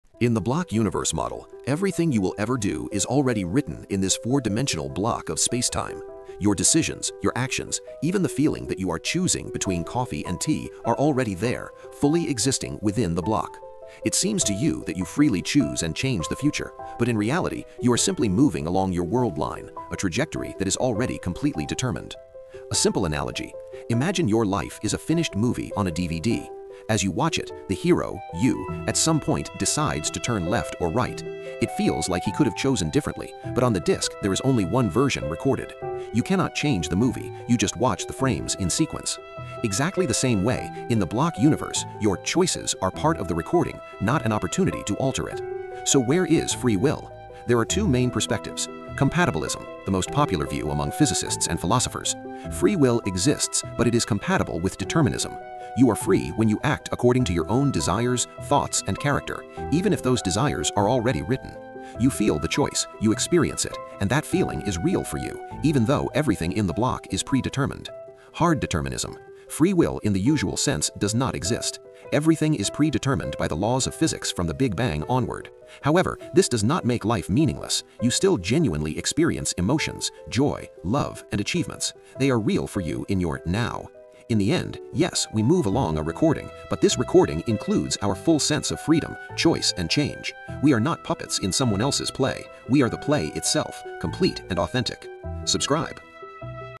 Is Time an Illusion?
In the block universe model—derived from Einstein's theory of relativity—past, present, and future all exist simultaneously in a four-dimensional block of spacetime.
Every decision you will ever make, every action, even the sensation of choosing between coffee or tea, is already fixed along your worldline.
Using a simple DVD movie analogy, we explore why time feels like it flows forward, yet may be completely static. We then dive into the question of free will:

Compatibilism: Free will is real and compatible with determinism—you are free when you act according to your own desires and character.
Hard determinism: Classical free will is an illusion, but life remains meaningful through genuine subjective experience.

This video breaks down one of the most profound ideas in modern physics and philosophy.
Watch to the end and share your thoughts: Does free will survive in the block universe?